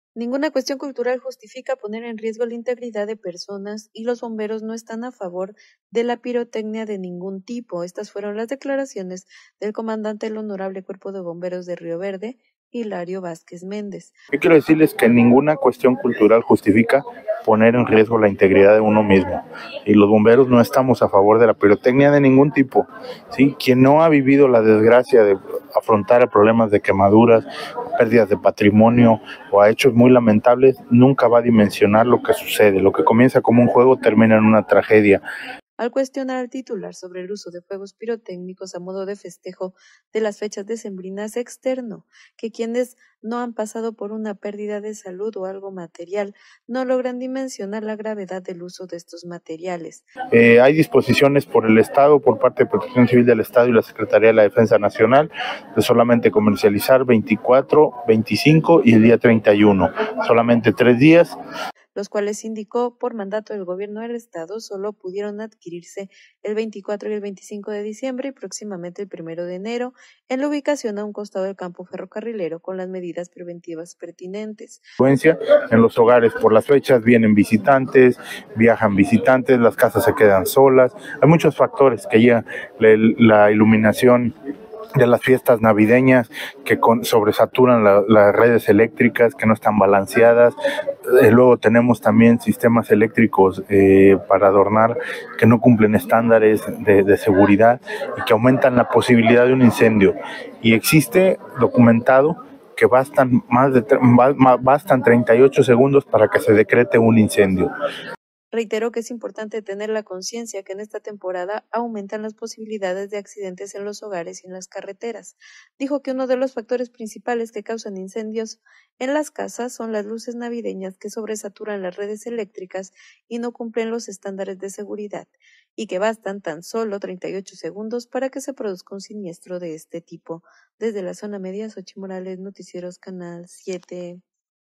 Tradiciones con pirotecnia, no justifican arriesgar la integridad: H. Cuerpo de Bomberos
Rioverde.-

Por: Xóchitl Morales